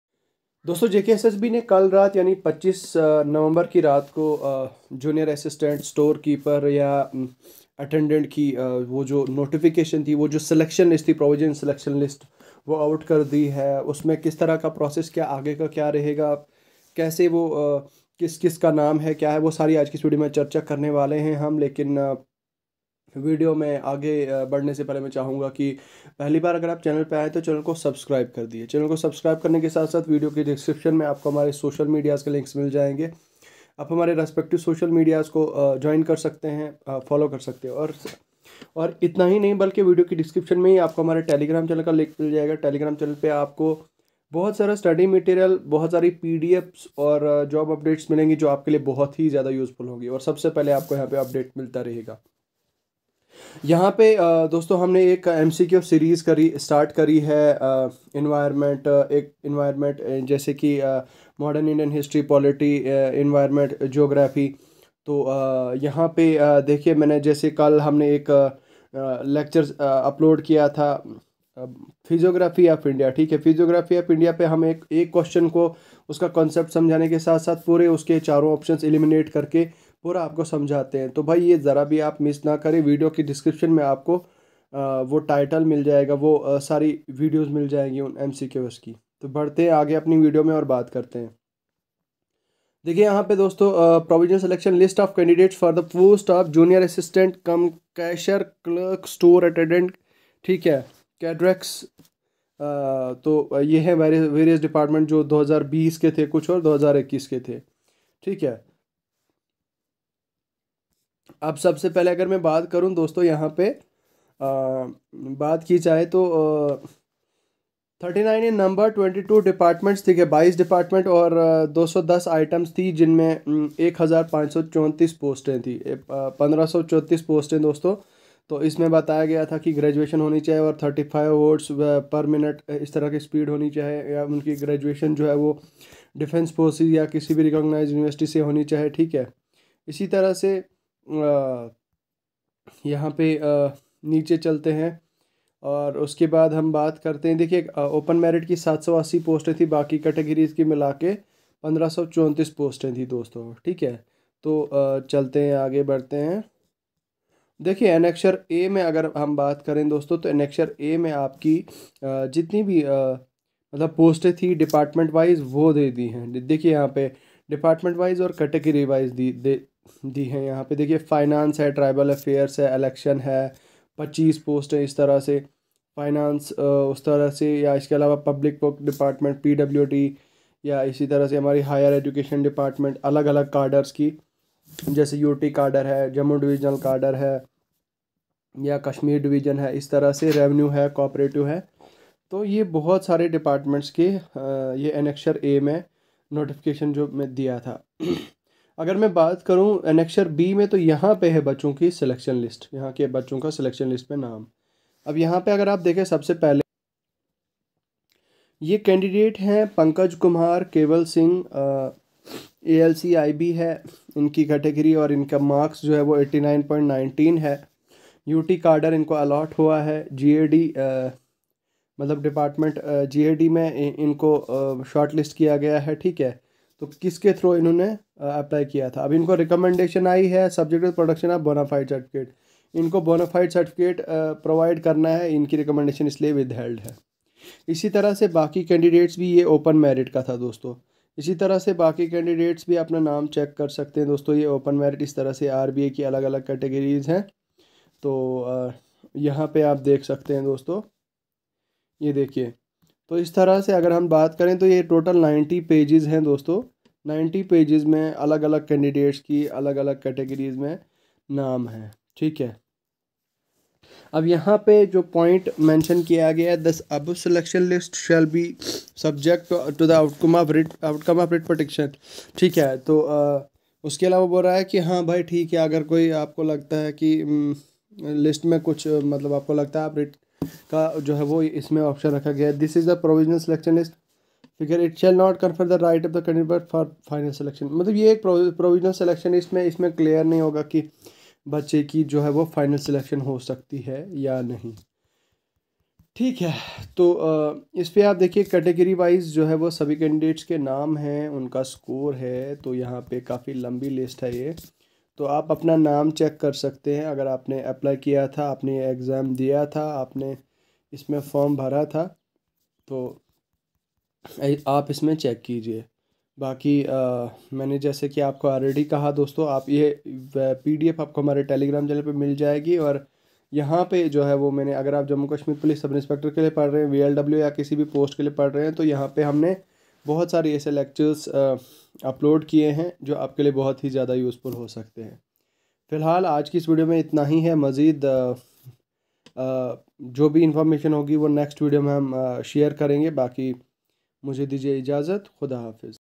JKssb New Selection List Out 🔥|Big Update|New Selection List For Junior Assistant 2020-21 Finally
The Exam Counsellor Presents you a New video on Jkssb New Selection List

For Further Updates and Information follow ous on our Following profiles ...